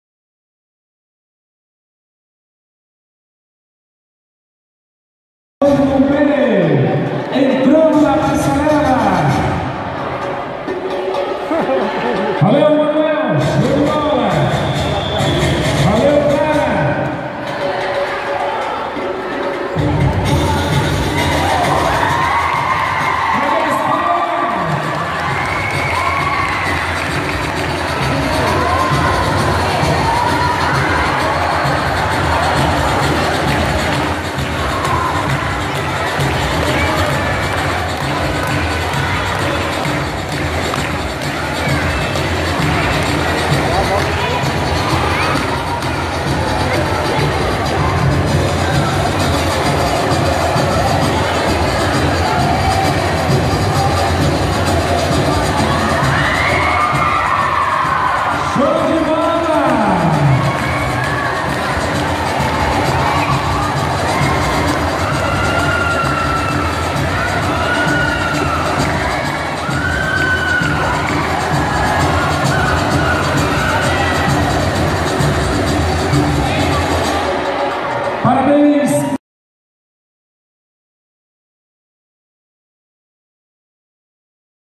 VICENFOLIA 2018 - I GINCANA CARNAVALESCA RELÂMPAGO/ ISJ
A Gincana Relâmpago Carnavalesca 2018 do Instituto São José tem o objetivo de promover a socialização, a integração e a interação dos alunos de forma lúdica; propiciando a estes, momento de prazer e alegria com responsabilidade, além de desenvolver a criatividade através da cultura popular.

Quinta Tarefa - "A equipe deverá apresentar uma candidata a "Musa do Carnaval 2018" e um candidato a"Rei do Carnaval 2018". Os candidatos serão avaliados pelos seguintes critérios: Elegância, simpatia, animação, fantasia e samba de pé.

Direção: Irmã Ana Piteira
Coordenadora: Magdalena Cavalcante

Nossos sinceros agradecimentos: Deus, Virgem da Medalha Milagrosa, Ir. Ana Piteira, Coordenadoras Pedagógicas: Mônica do Carmo e Magdalena Cavalcante, Funcionários, Professores responsáveis pelas turmas, Pais, Banda ISJ, Atila Cavalcante, Adriano Ponciano, Gerlúcio Giló, Assis Silva, Marciano Ponciano, Aldisia, Ocivan Moreira, Edivan Varelo, Silvanise Ponciano e aos nosso queridos alunos que abrilhantaram o VICENFOLIA 2018.

6º Ano B em Ação!